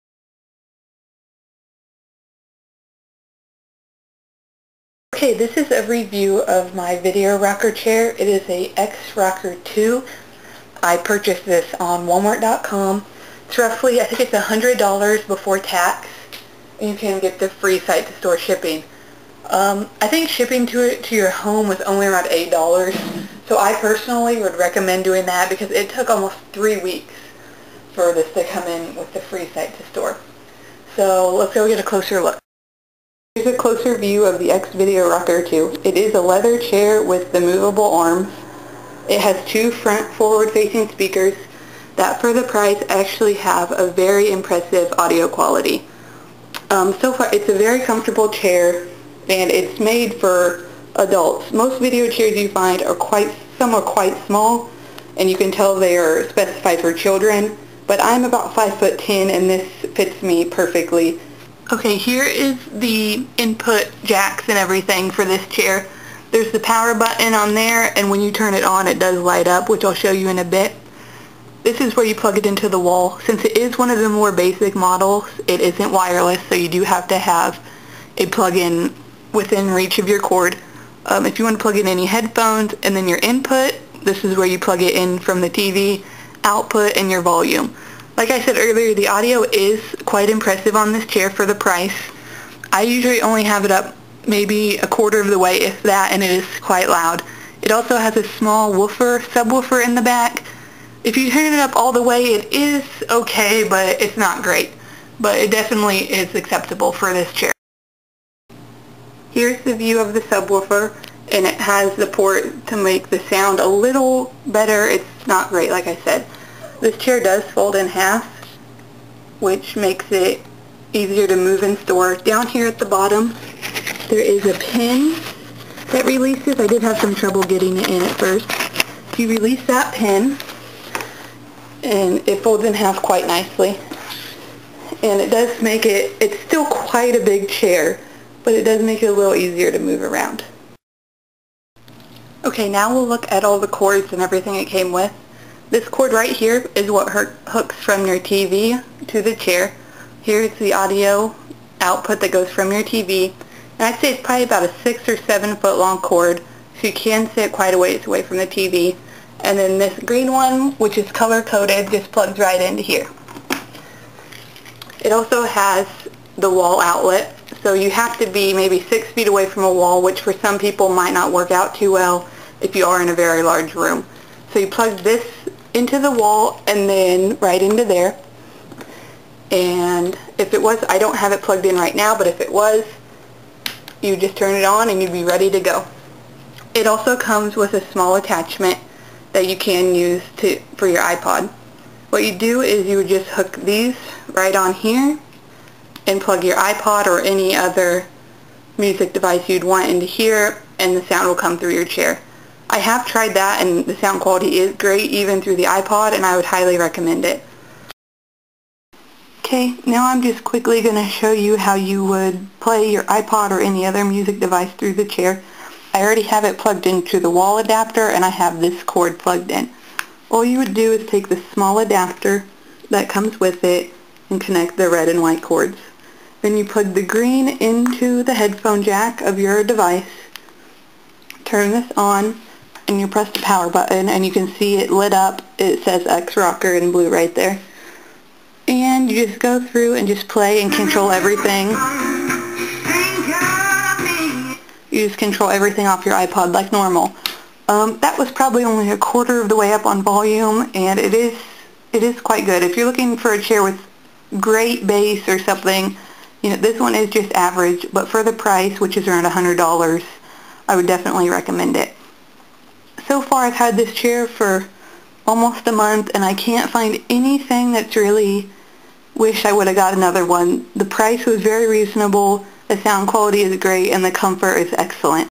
X Video Rocker II Review
This is a review of the gaming chair, X Video Rocker II. I purchased this chair from walmart.com for $100, and it is great for a lower priced chair. I would definitely recommend it. Please feel free to post any comments or questions you may have. Any ratings and subscriptions would also be greatly appreciated!